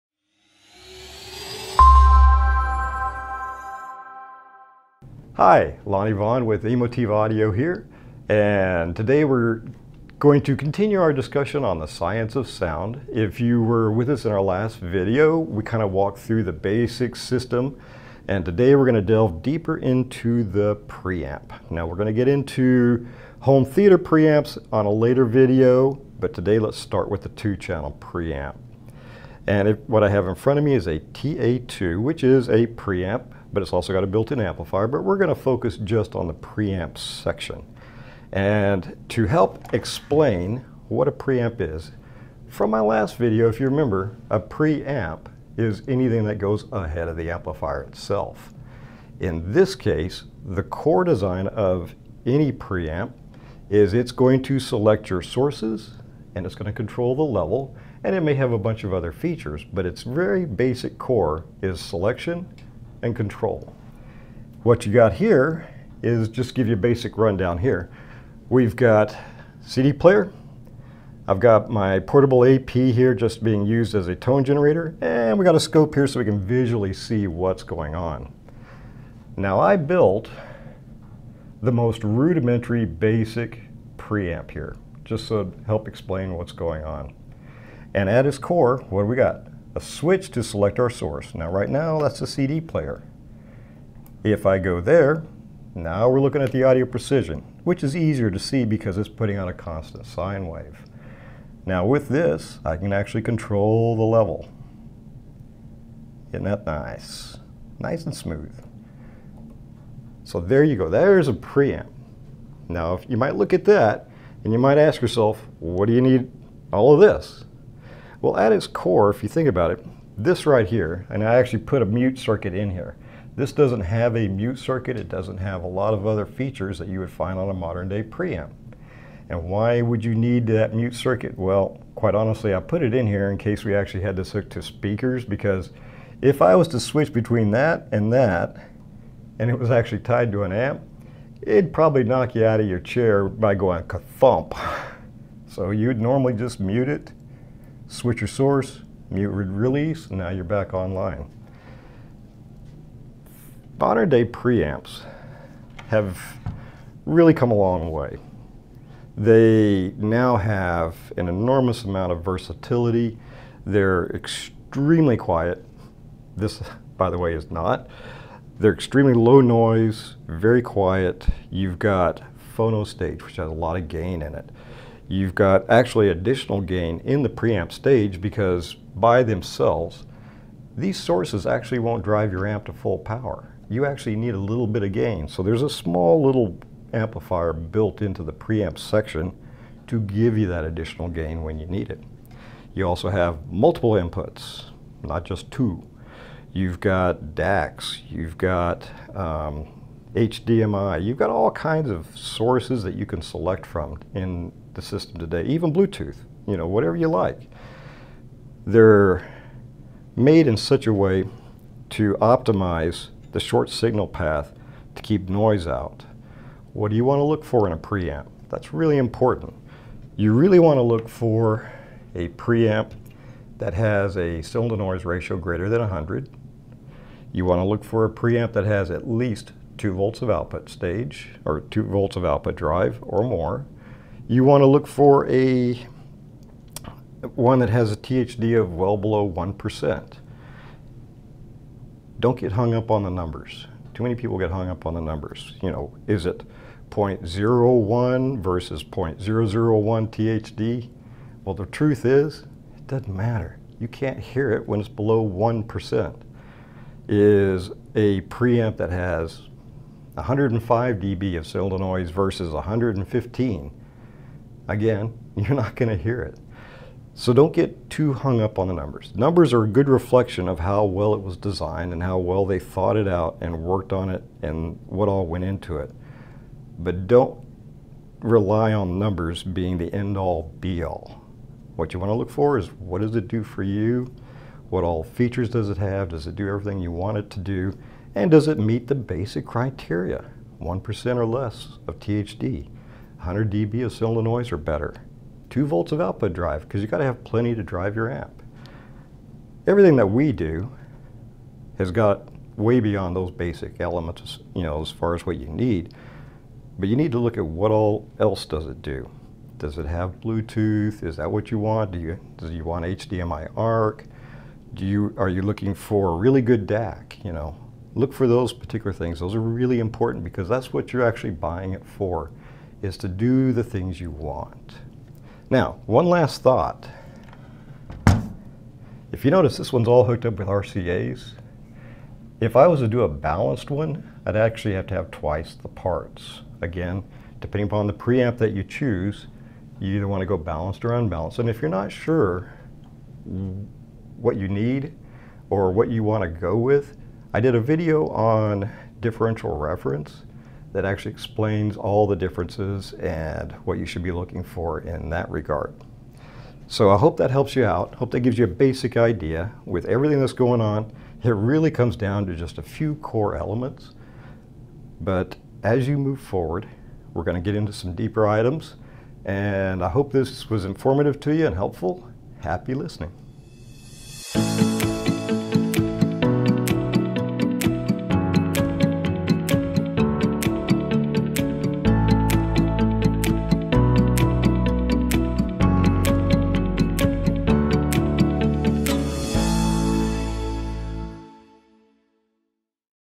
How does a Preamplifier work?
Our very own Lonnie has an in depth discussion about the ins and outs of stereo preamplifiers.